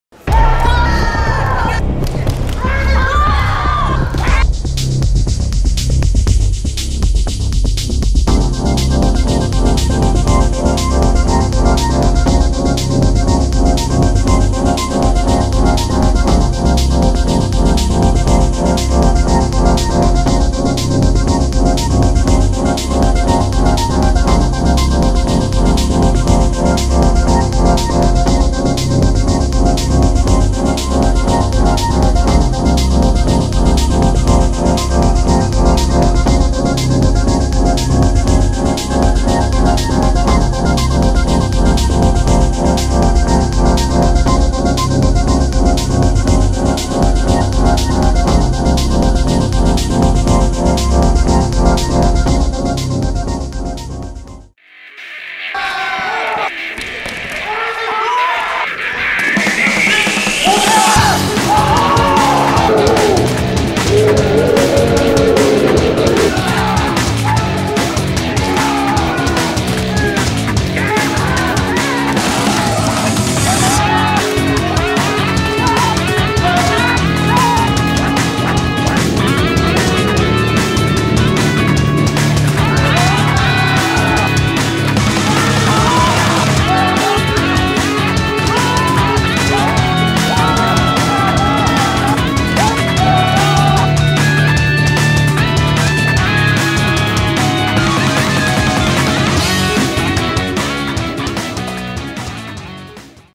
最強のガードマン決定戦！？綜警防護術大会がすごい！！
ALSOKの社技のひとつ、「綜警防護術」の紹介動画です。
多彩な技が出される試合の模様を是非ご覧下さい。